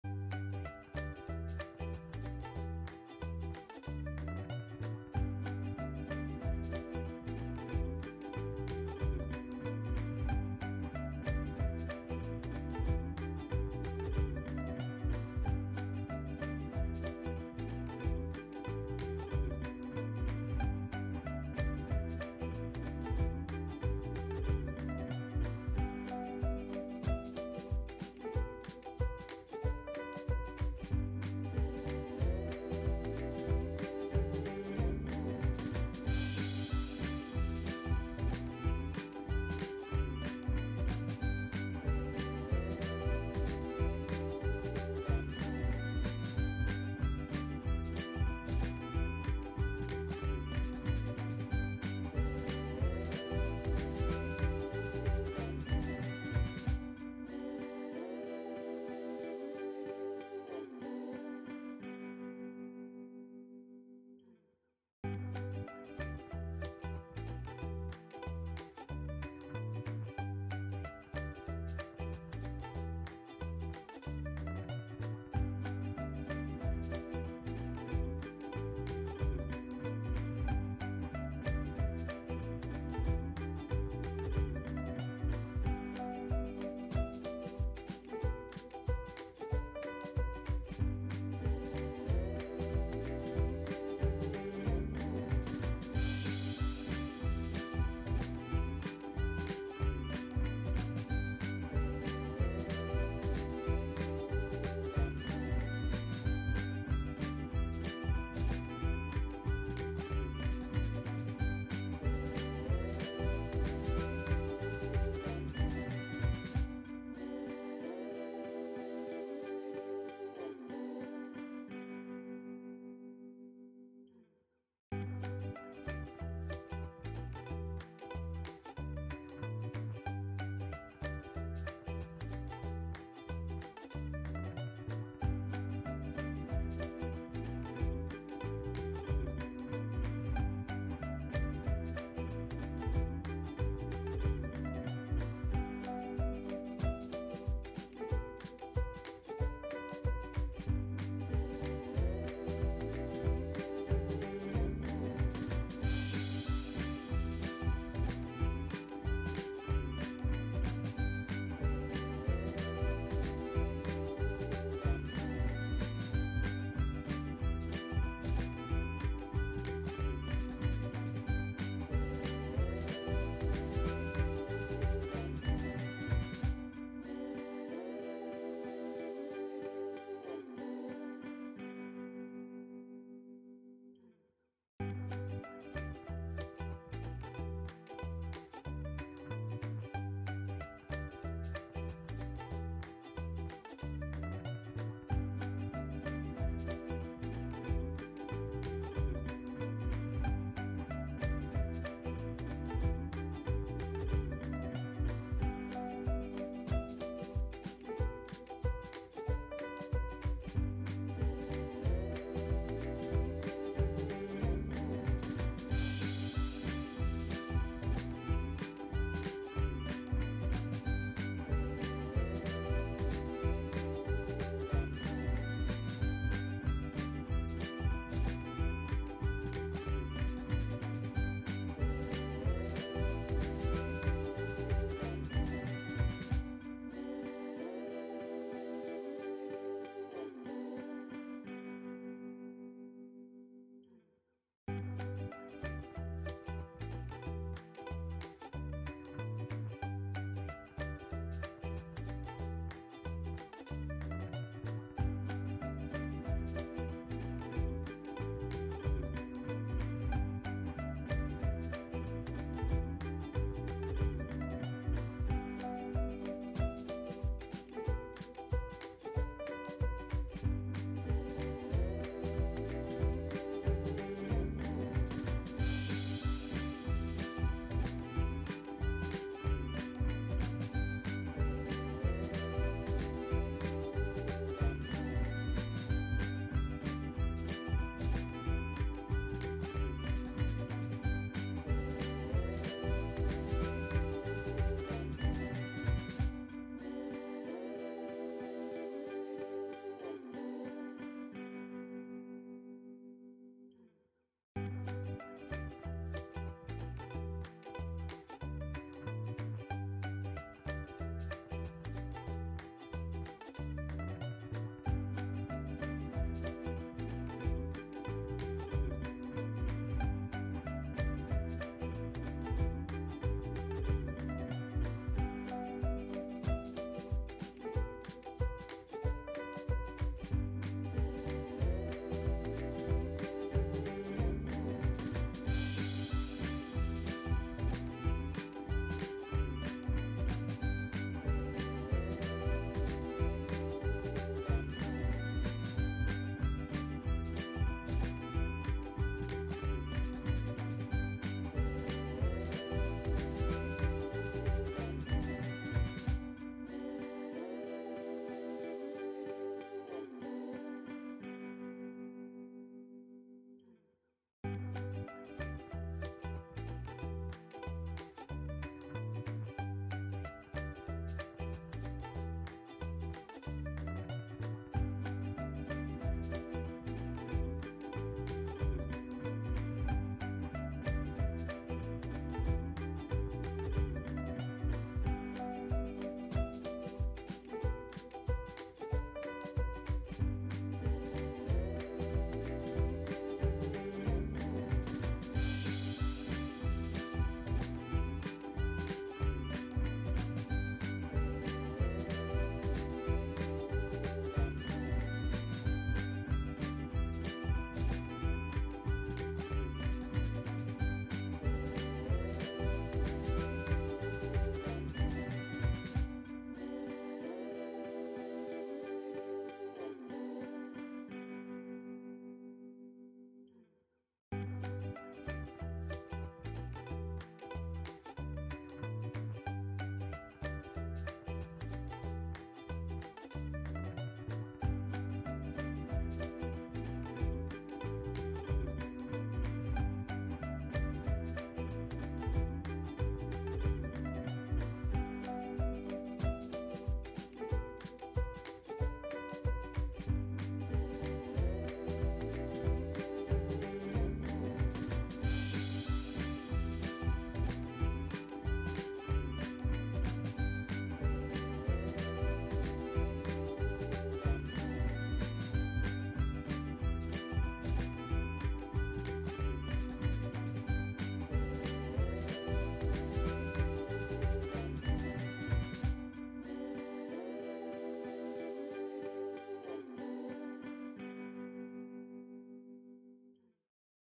🥰 Turshia me Speca të Kuqë të Pjekura, Pa Konzervanse, Receta më e Shijshme 🌶❤️
Përbërsit :
7 kg Speca të Kuqë
Majdanoz
Hudhër
300 ml Vaj
1 lugë gjelle Sheqer
Kripë
20 ml Uthëll
Ingredients :
7 kg of Red Peppers
Parsley
Garlic
300 ml of Oil
1 soup spoon of Sugar
Salt
20 ml of Vinegar